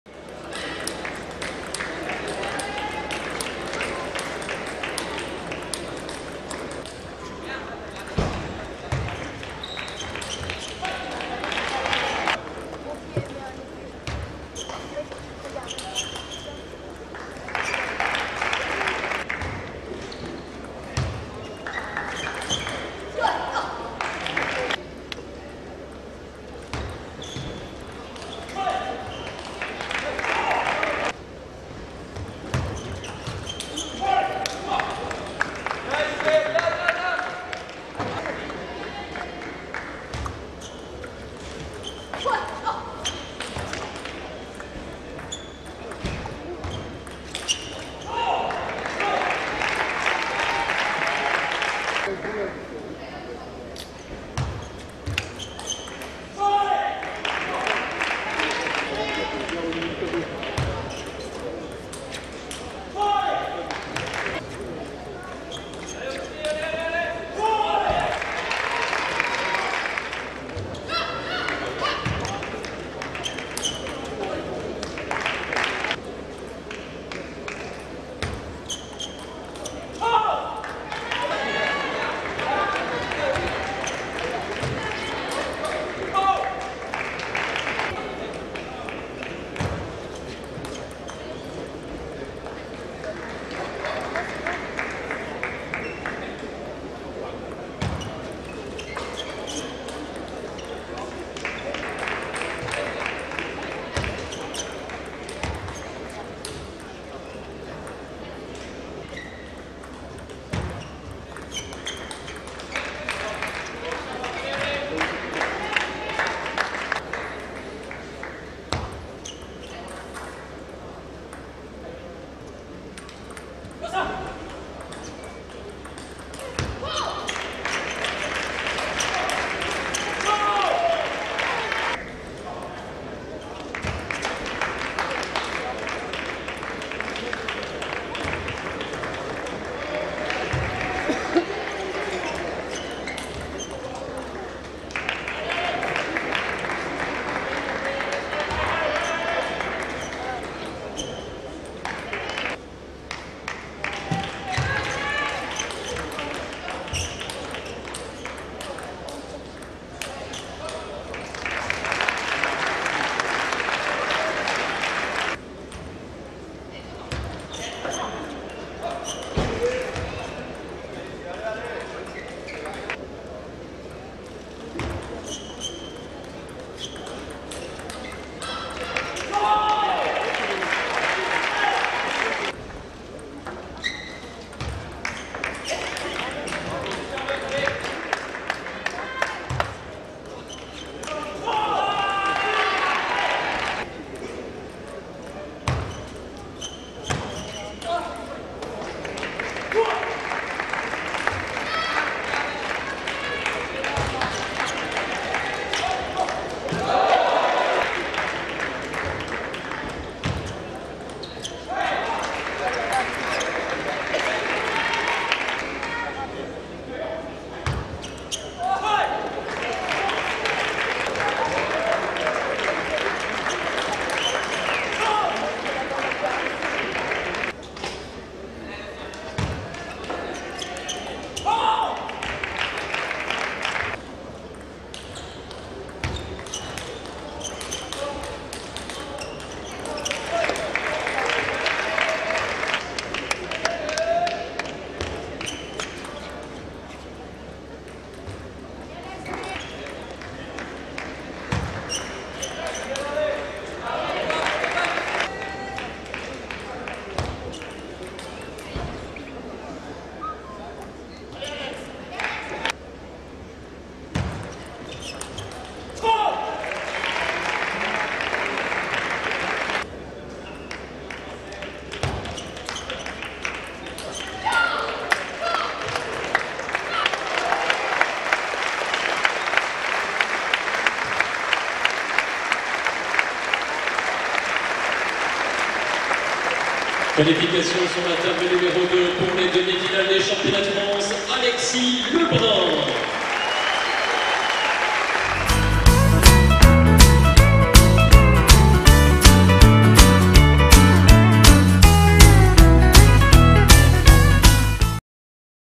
Alexis LEBRUN - Esteban DORR (France 1023)
Extraits 1/4 finale championnat de France seniors masculins à Antibes